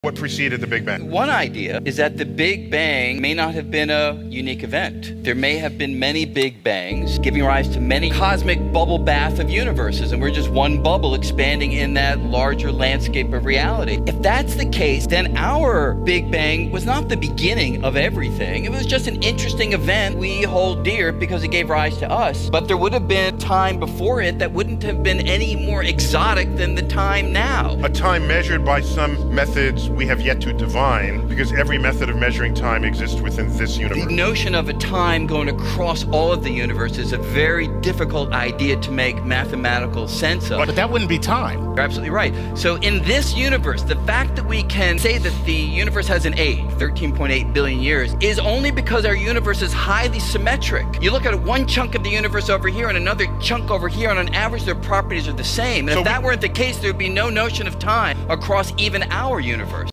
Multiple Big Bangs 🤯 w/ Neil Degrasse Tyson
Dive into the mind-bending theory of multiple Big Bangs with Neil deGrasse Tyson, as he navigates the cosmos to explore the possibility of endless universe creation. Imagine a scenario where our Big Bang is just one of many, with countless others sparking into existence continuously. Neil elucidates this concept, suggesting that the universe we know is merely a bubble in an infinite sea of cosmic expansions. With his unique ability to blend complex science with engaging narrative, Neil makes the idea of multiple Big Bangs not just comprehensible, but utterly captivating. Join him as he ventures through the theoretical physics, the observable evidence, and the sheer wonder of a multiverse constantly in flux.